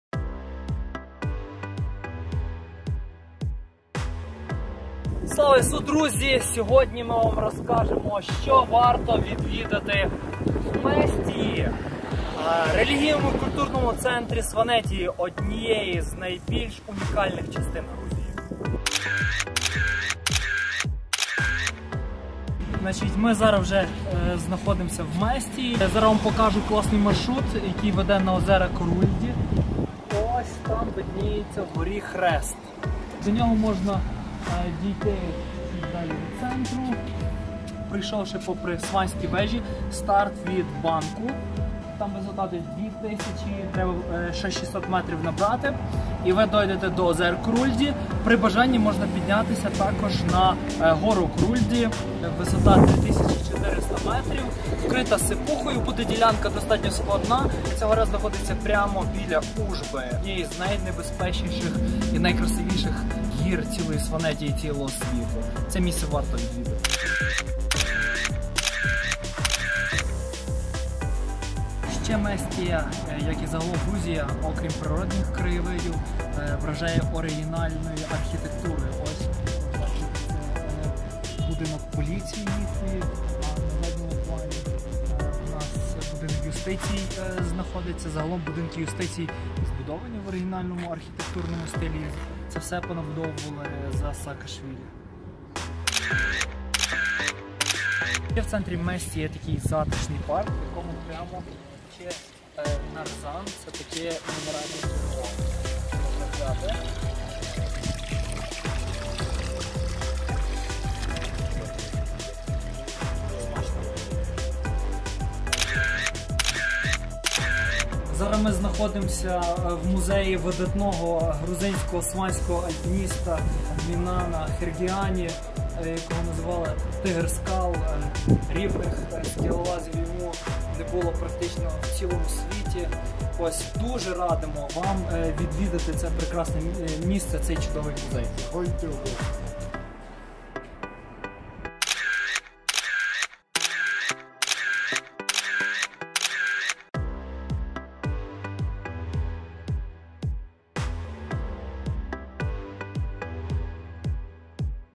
Екскурсія по Местії І Столиця Сванетії
Местія - столиця Сванетії.

Що варто відвідати в серці Сванетії? Куди йти? Як добратися до озер Корульді?

Відповіді на всі ці запитання в нашому відео.

💰Приватбанк: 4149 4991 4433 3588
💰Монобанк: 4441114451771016